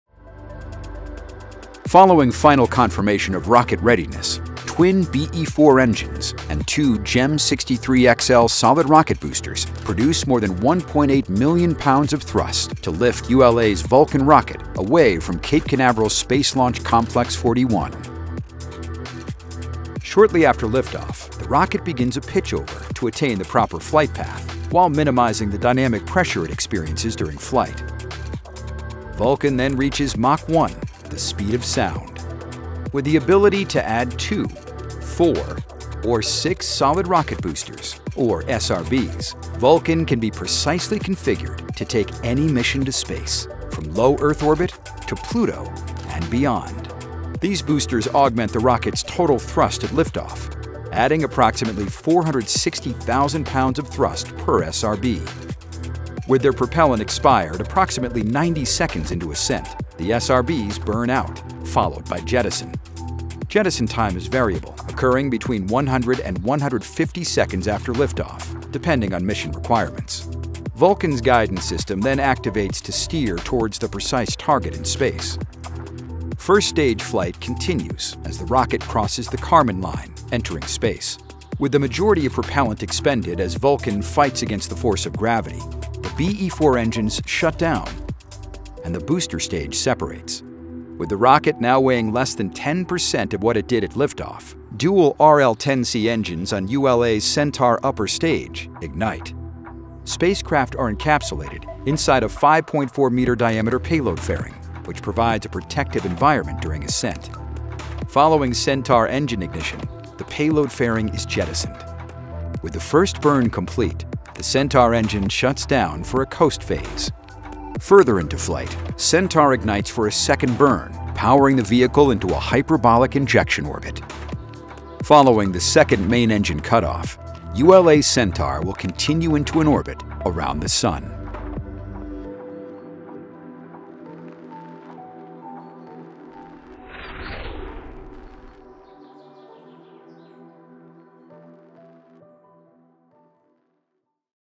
Vulcan's Second Flight: Earth to Orbit
Earth to Orbit: A United Launch Alliance (ULA) Vulcan VC2S rocket will launch the second certification (Cert-2) mission from Space Launch Complex-41 at Cape Canaveral Space Force Station, Florida. The Cert-2 mission includes an inert payload and demonstrations associated with future Centaur V technologies. This is the second of two test flights required for ULA's certification process with the U.S. Space Force.